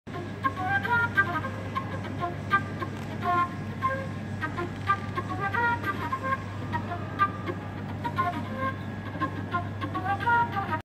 Night Sky Secrets Anti-gravitational
Anti-gravitational Revolving Speaker - seen here at café Cheeky Yam. Powerful speaker with loads of volume and cool factor! 😜